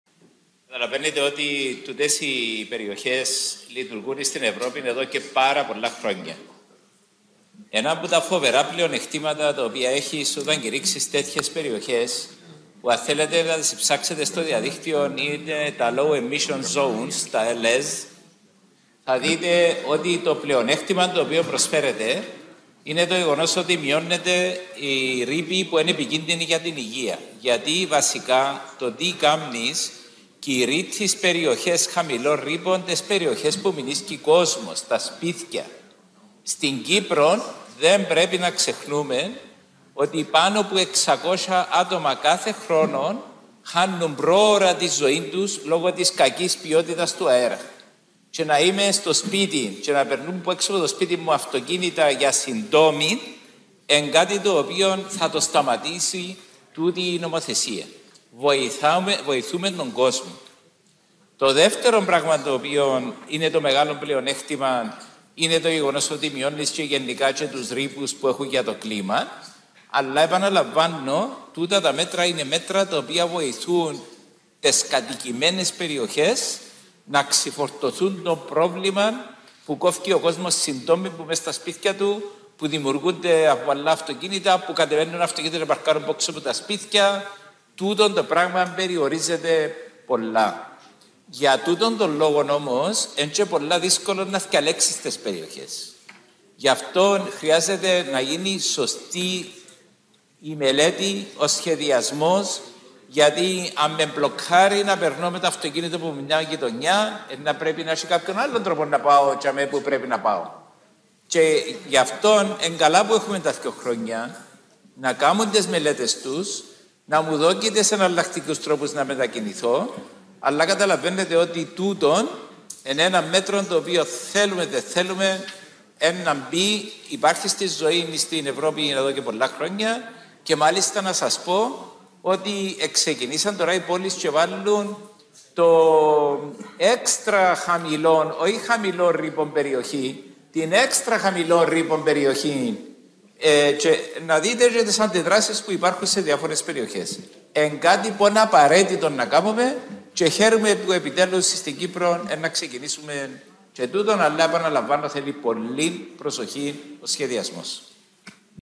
02/11/23 - Τοποθέτηση Χαράλαμπου Θεοπέμπτου στην ολομέλεια της Βουλής Low Emissions Zones
Ο Βουλευτής του Κινήματος κ. Χαράλαμπος Θεοπέμπτου, μιλώντας για τα Low Emissions Zones, εξήγησε ότι στην Κύπρο χάνουν τη ζωή τους περίπου 600 άνθρωποι τον χρόνο λόγω της κακής ποιότητας του αέρα. Απαραίτητο μέτρο αλλά θέλει πολλή προσοχή ο σχεδιασμός του.